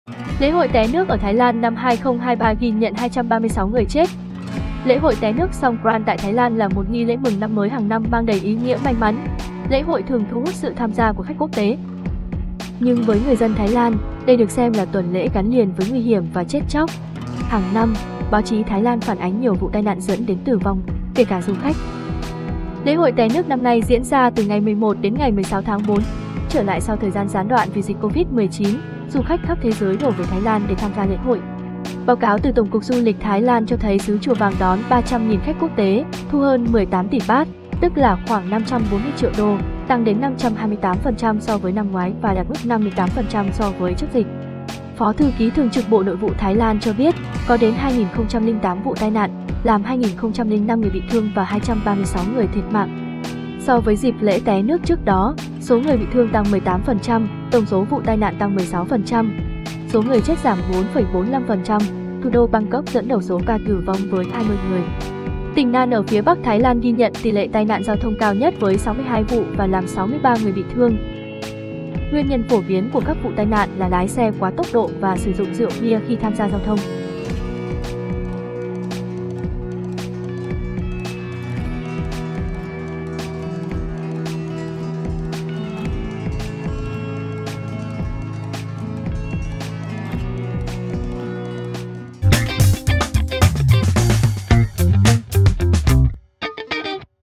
Lễ hội té nước Songkran tại Thái Lan có 236 người c.h.ế.t
✥ Lễ hội té nước Songkran diễn ra từ 11-16/4. Trở lại sau thời gian gián đoạn vì dịch COVID-19, du khách khắp thế giới đổ về Thái Lan để tham gia lễ hội.